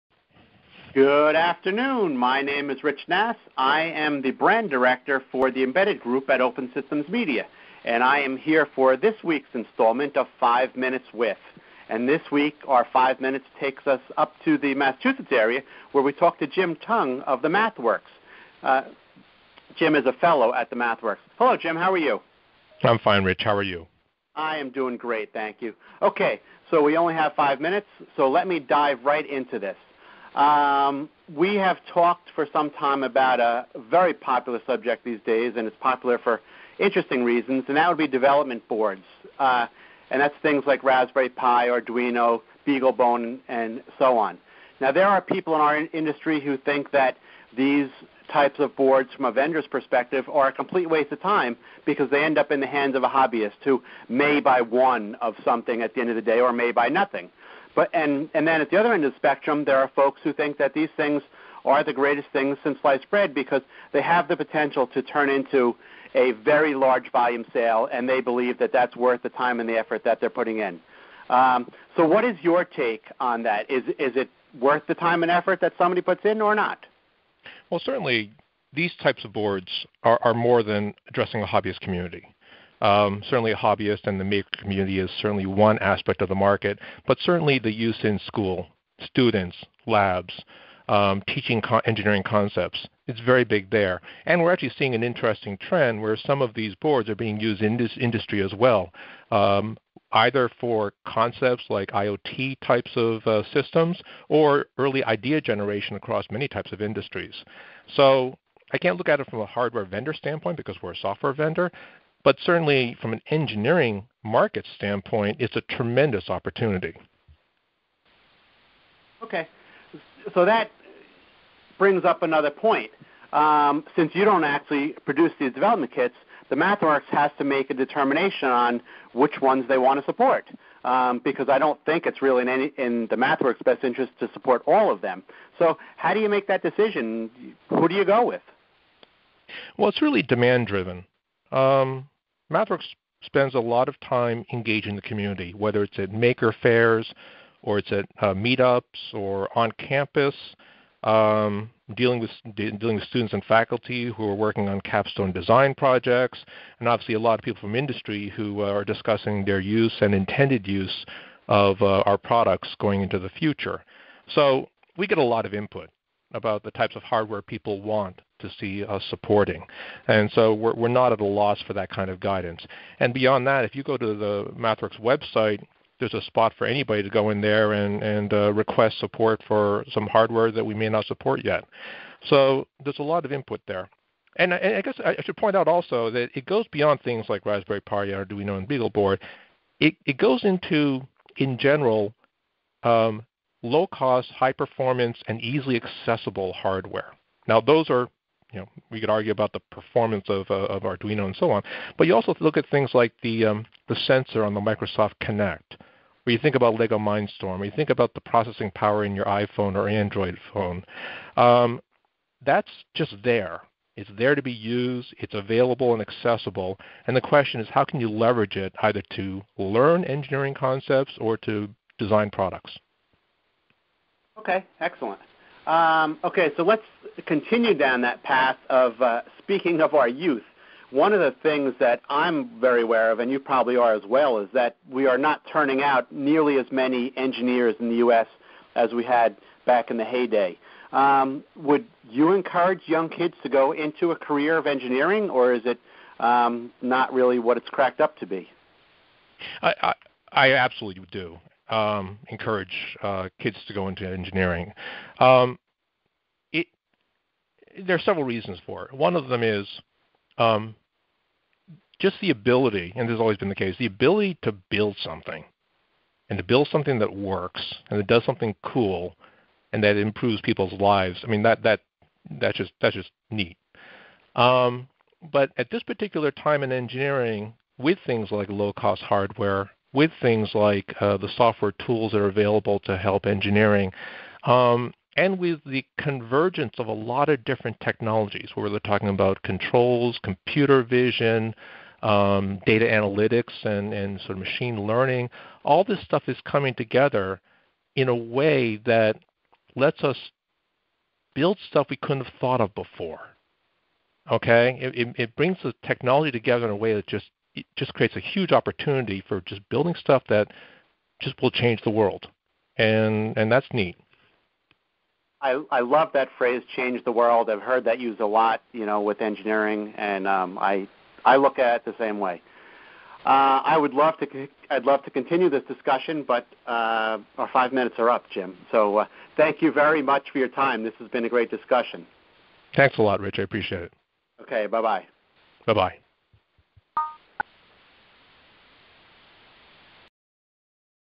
Five Minutes with Jim Tung, Fellow, MathWorks
We cover a lot of development kits on our site (embedded-computing.com). Some people think they're a waste of time, while others think we can no longer live without them. I asked Jim Tung, a Fellow at The MathWorks, his opinion on the value of these kits, as they're a company that makes a significant investment in these products. One important aspect, from a software company's perspective is, which development kit do I support? Finally, we discussed whether engineering is a "glamorous" profession. Jim had a great response to that one.